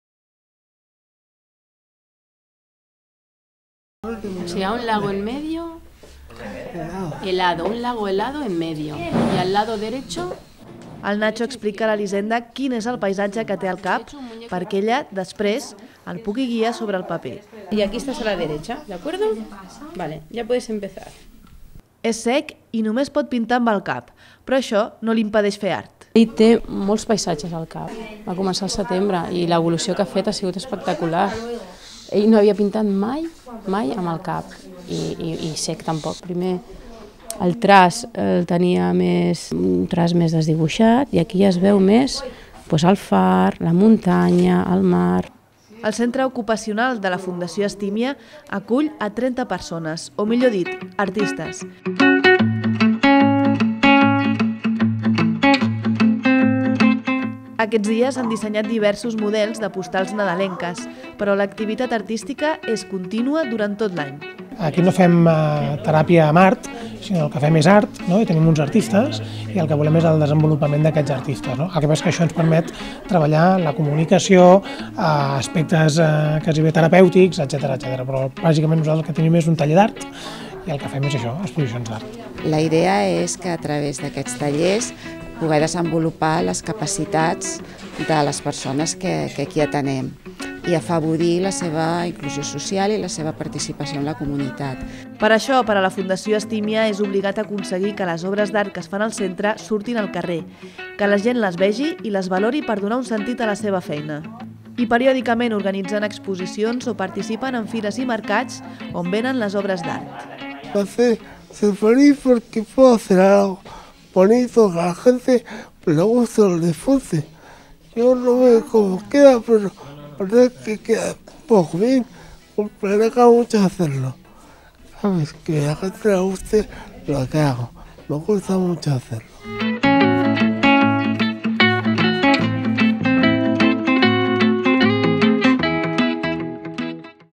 Els artistes de la Fundació Estimia
A la Fundació Estimia ajuden a persones discapacitades a fer créixer la seva vessant artística. Aconsegueixen obres d'art magnífiques que després exposen i venen. D'aquesta manera donen sentit a la feina que fan les 30 persones que acull aquest centre ocupacional. Van començar als anys 80 i des de llavors han treballat tota mena de creacions artístiques buscant la manera d'adaptar-les a qualsevol tipus de discapacitat.